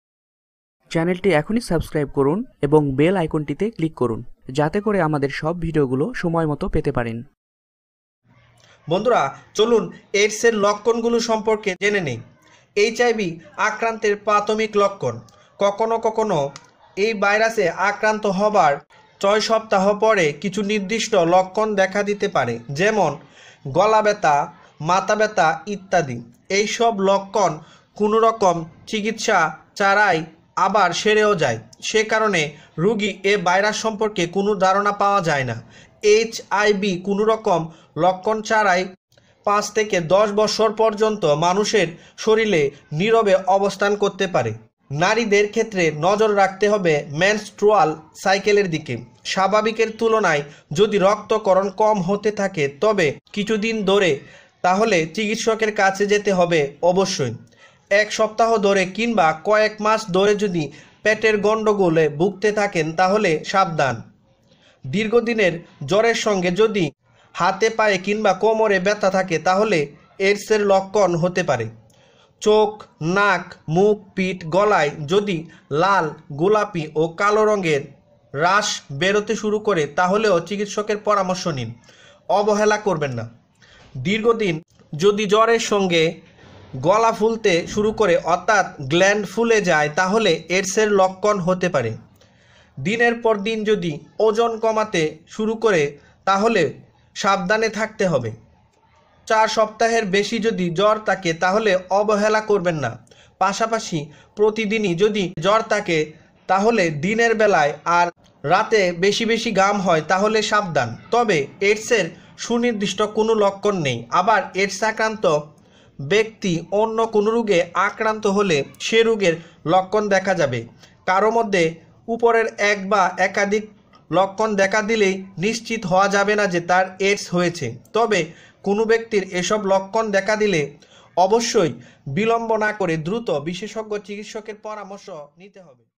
AIDS/HIV এর প্রধান লক্ষনসমুহ | Symptoms of AIDS/HIV | Bangla Health Tips
AIDS/HIV is very Dangerous disease and  suffering this diseases person will die very fast. In this video I am going share with you some symptoms of AIDS /HIV diseases. Hope this information will make you more sincere about this vital matters.